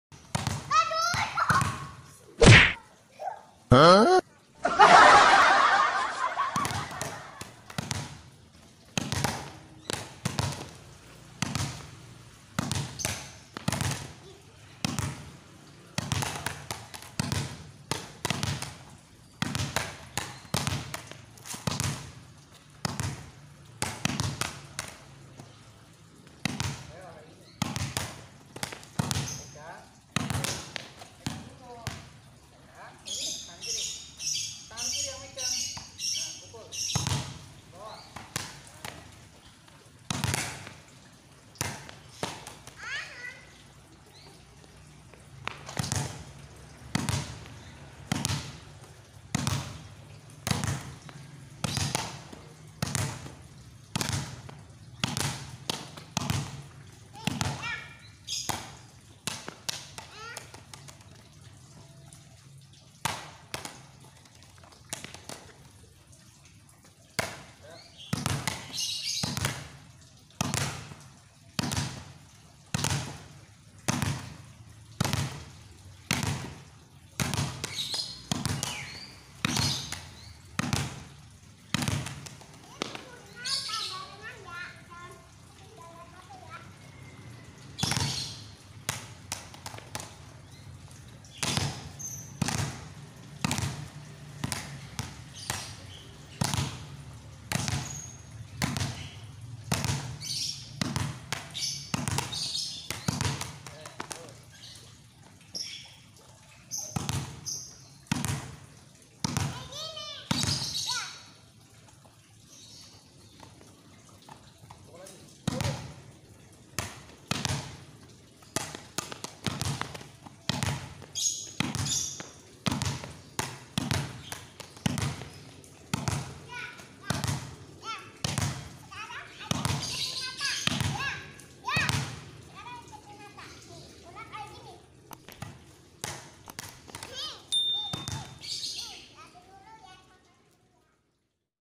Cara Melatih Poles Tangan Bola Voli Pemula Dengan Memukul Bola Ke Lantai!!!
Cara melakukan atau latihan poles tangan bola voli agar pukulan bagus dan baik...
Lakukan dengan rutin agar hasil maksimal dan memuaskan .